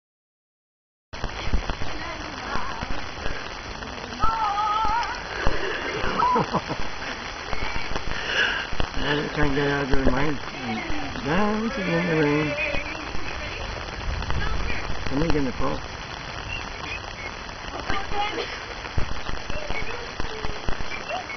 Dancing in the rain
Lauren and Amanda ..well,dancing in the rain...